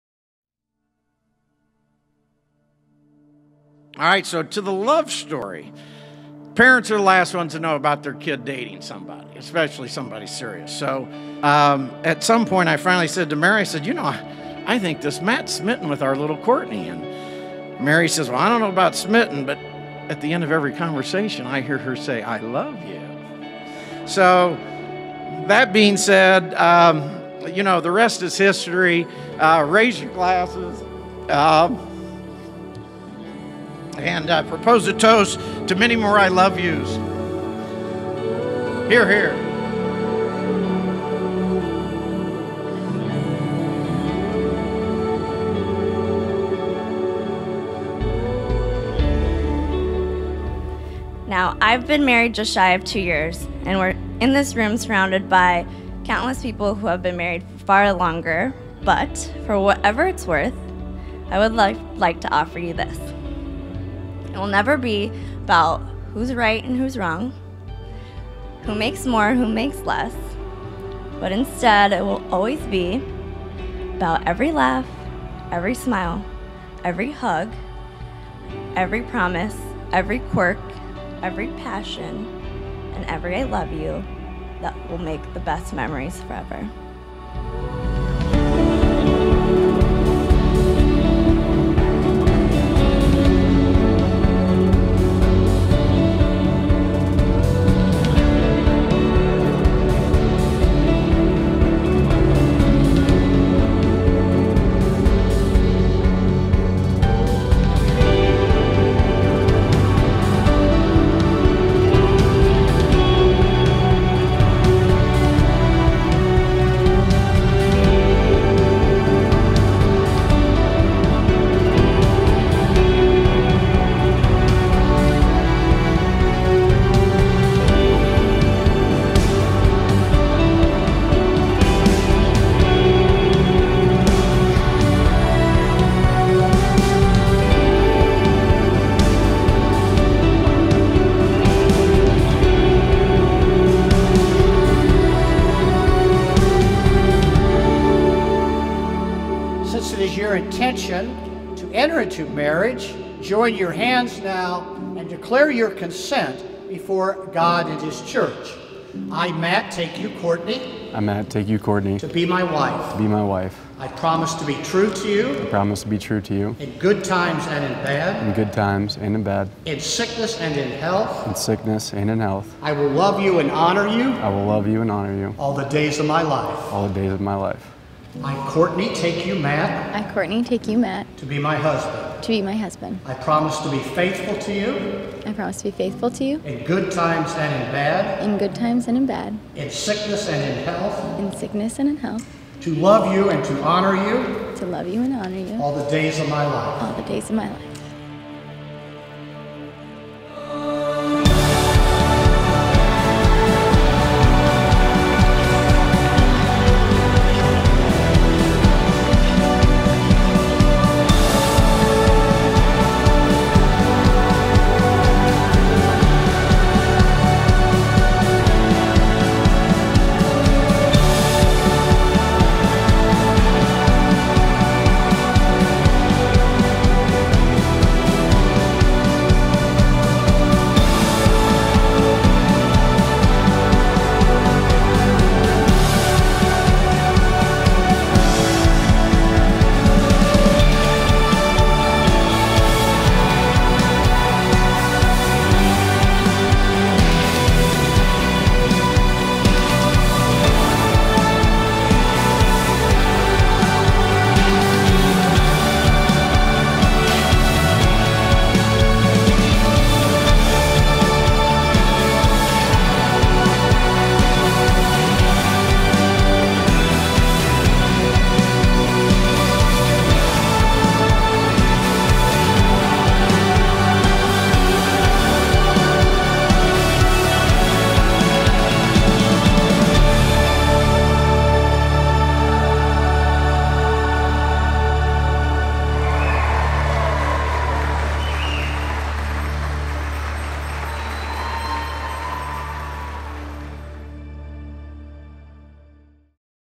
Courtney & Matt's Wedding at The Indiana Roof Ballroom
Courtney & Matt's Indiana Roof Ballroom Wedding in Indianapolis, Indiana. A Vibrant Video Production.